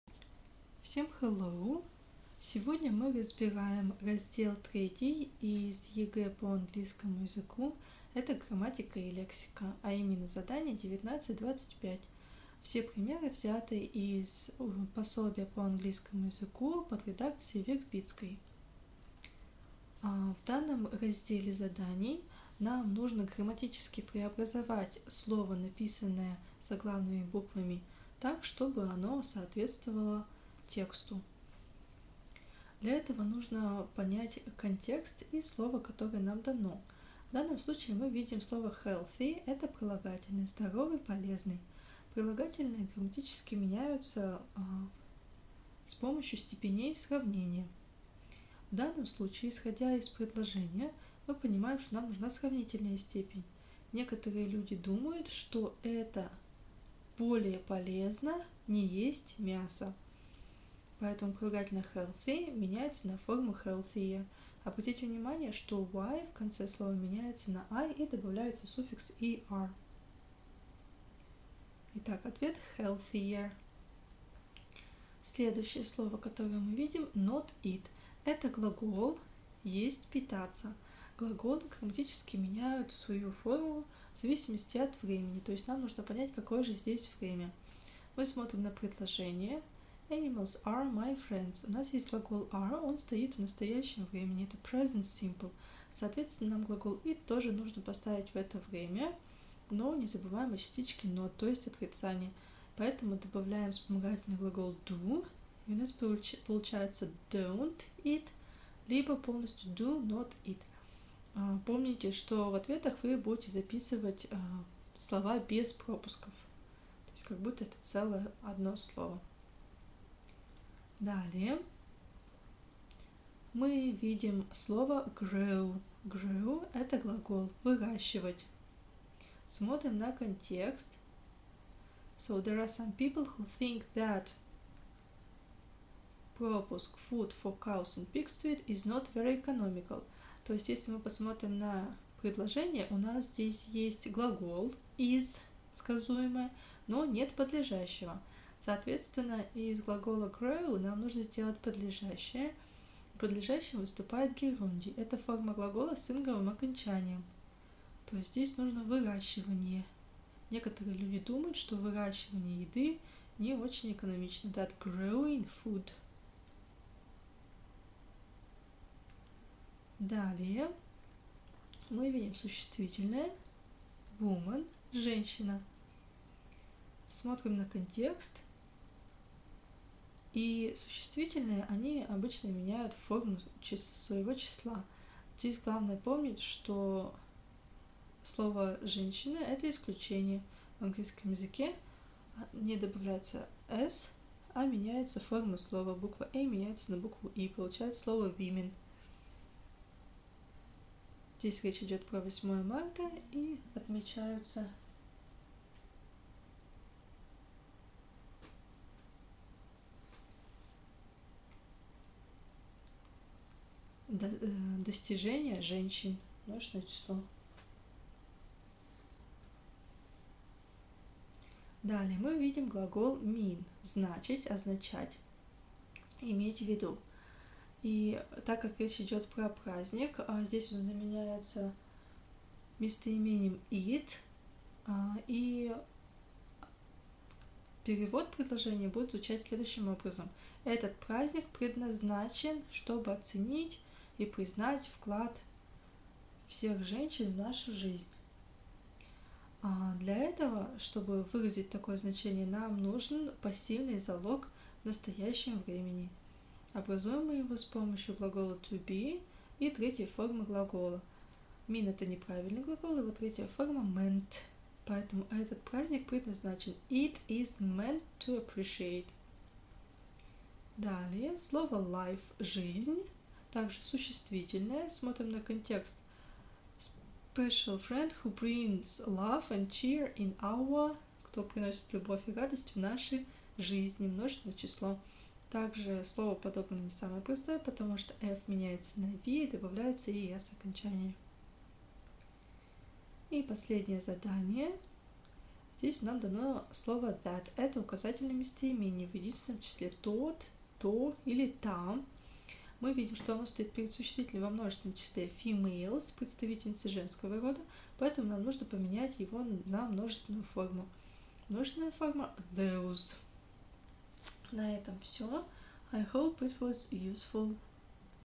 ЕГЭ Английский язык Грамматика и Лексика Задания 19-25
Разбираем грамматические формы слов в разделе 3 ЕГЭ по английскому языку